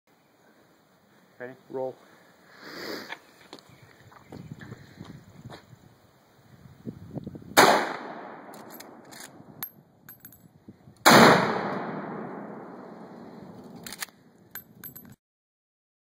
0.5 pounds of Tannerite
Experimenting with Tannerite for the first time.  Bought 0.5 lbs of it at Gander Mountain for $8.  Shot it with my Winchester 94, caliber 30-30.